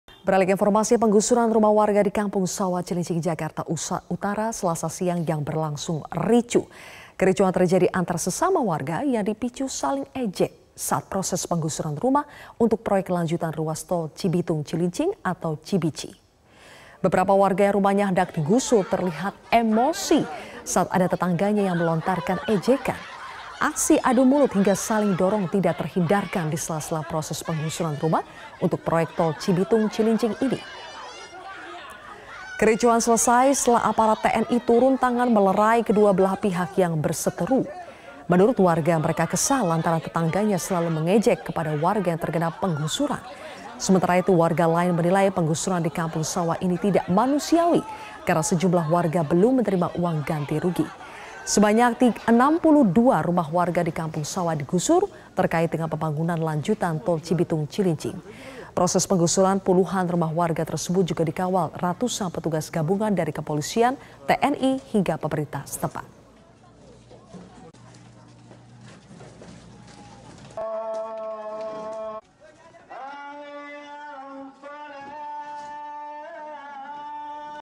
Korban Penggusuran Emosi Diejek Tetangga
MetroTV, Penggusuran rumah warga di Cilincing, Jakarta Utara, berlangsung ricuh, Selasa (8/11/2022). Kericuhan terjadi antar warga yang dipicu saling ejek saat proses penggusuran rumah untuk proyek lanjutan ruas Tol Cibitung-Cilincing (Cibici).

Aksi saling dorong dan adu mulut tak terhindarkan sela-sela penggusuran rumah. Menurut warga, mereka kesal karena tetangganya selalu mengejek kepada warga yang terkena penggusuran. Sementara itu,warga lain menilai penggusuran ini tidak manusiawi karena sejumlah warga belum menerima uang ganti rugi.